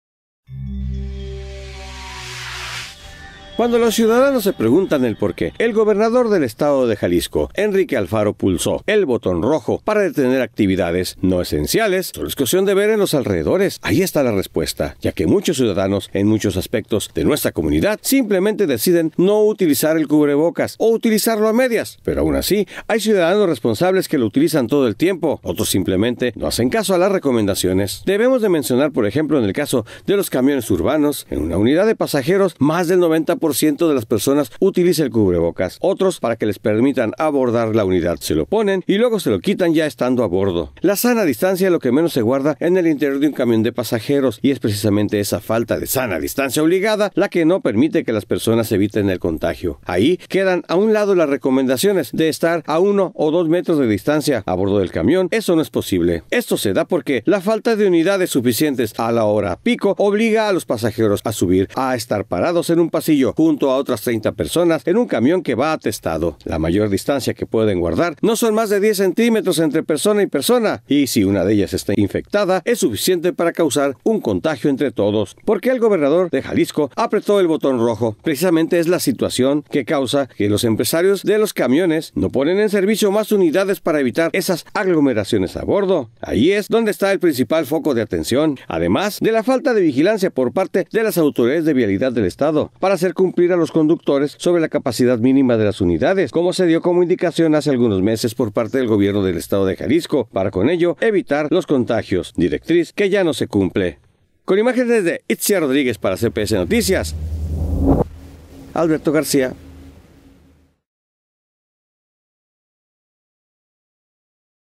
Paradas de autobús, se han convertido en uno de los principales focos de contagio de COVID 19
Las unidades de transporte público, así como las paradas de autobús, se han convertido en uno de los principales focos de contagio de COVID 19, pues en ellos se aglomeran las personas, sin acatar la recomendación de la sana distancia.